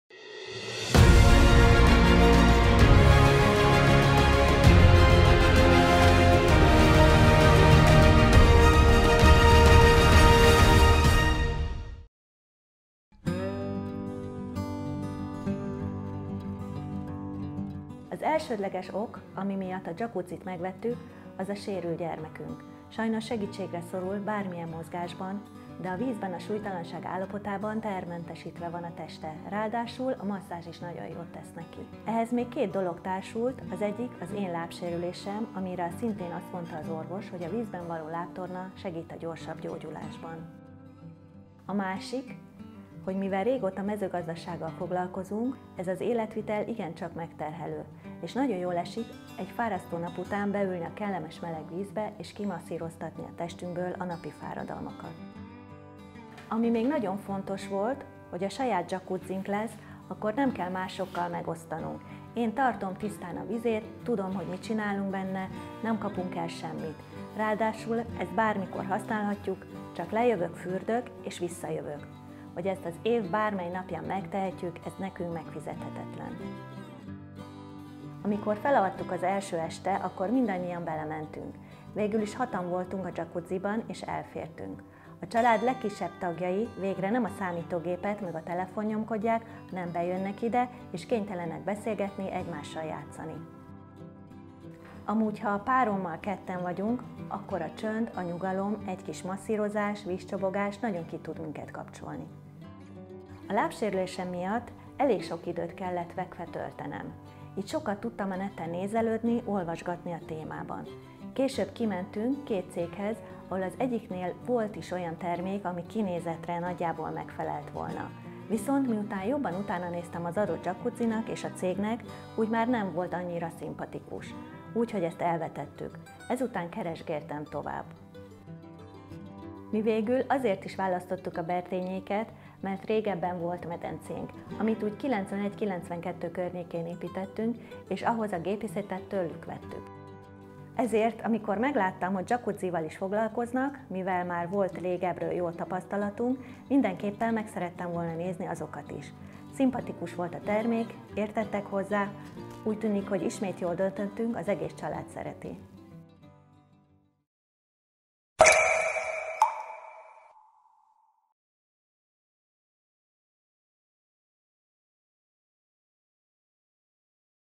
Jacuzzi vélemény tapasztalat - Neptun jacuzzi
Egy szimpatikus hölgy keresett meg minket jacuzzival kapcsolatban. Nem volt olyan jó véleménye azokról a termékekről és cégekről amiket eddig látott, és mint kiderült, mivel 26 évvel ezelőtt már volt pozitív tapasztalata velünk kapcsolatban, így nagyon megörült mikor megtudta, hogy mi is forgalmazunk jacuzzikat.

Számára az volt a fő szempont ami miatt jacuzzit szeretett volna a saját és párja otthonába, hogy a gyermekük, aki sajnos sérült, el tudjon lazulni a vízben, mert amúgy nem tud mozogni. Csak ezután jöttek a szülői igények.

Mivel az év jelentős részét mezőgazdasággal töltik, ami egy igen kemény és kevés pihenést megengedő munka, ezért szerették volna azt a kis időt amit magukra szánhatnak a lehető legkényelmesebben, jó hangulatban, idegenektől távol, zaj és stresszmenetesen eltölteni.

Miután kiválasztottuk a számukra ideális terméket, ami egyébként a kínálatunkból a 6 személyes Neptun jacuzzi lett, már alig várták, hogy telepítsük és végre az egész család birtokba vehesse.

Mint megtudtuk, az első alkalommal, aznap mikor beüzemeltük a jacuzzit, a család mind a 6 tagja egyszerre ült benne és mindenki nagyon jól érezte magát.

Reméljük hosszú - hosszú évekig legalább ilyen jó hasznát fogják venni a jacuzzinak.